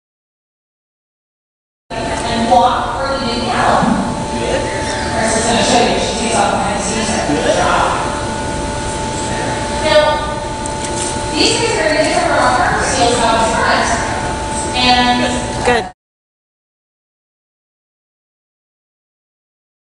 New Balance Foundation Fitness Friday - Kick, spin and shake with Isaac
Isaac the fur seal works with Paul the marine mammal trainer to shake, kick and spin his way to fitness. Learn more about New Balance Foundation Fitness Fridays on the Trainers Blog